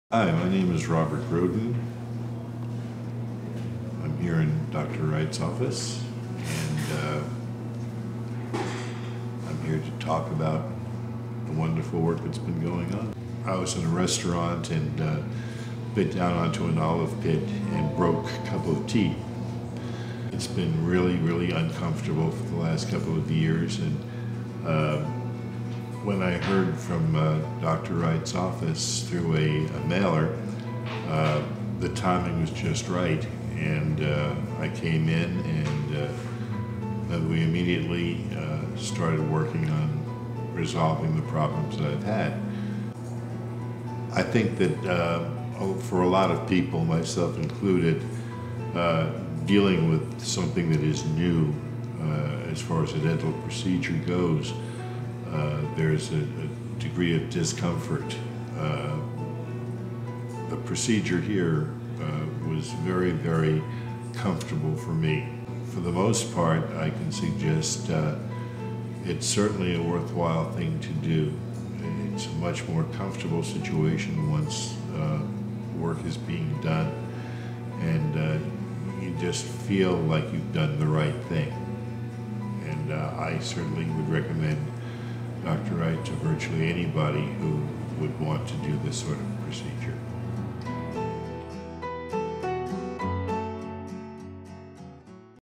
Robert's Oral Surgery Implant Dentures | Just Wright Dental
Educated and informed patients make the best decisions.   We do not sell dentistry, but via co-discovery and trust make a mutual decision that fits within the patients budget to help them obtain the best available result.   Robert's story is wonderful like many others we have been able to help in very few appointments and promptly.